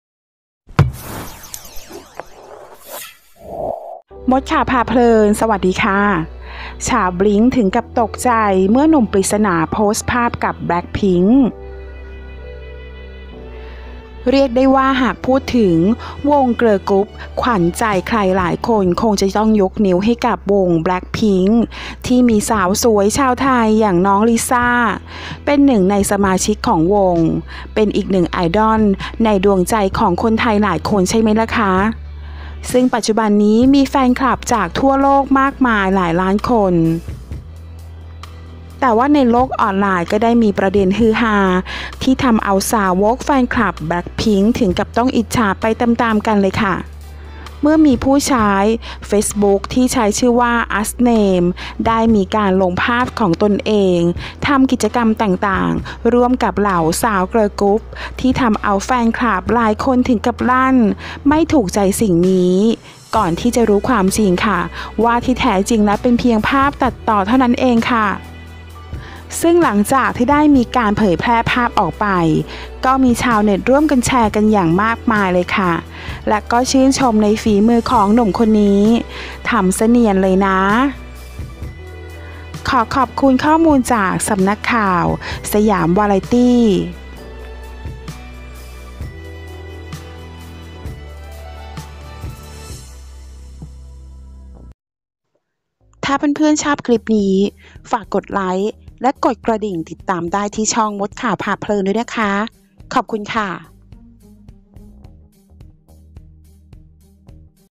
ชาวบลิ้ง ถึงกับตกใจ เมื่อหนุ่มปริศนา โพสต์ภาพกับ blackpink
หนุ่มโพสต์ภาพเปิดตัว ทำสาวก blackpink เจ็บจิ๊ด
ช
าวบลิ้ง ถึงกับตกใจ เมื่อหนุ่มปริศนา โพสต์ภาพกับ blackpink

ขอขอบคุณแหล่งข่าวข้อมูลจากสำนักข่าว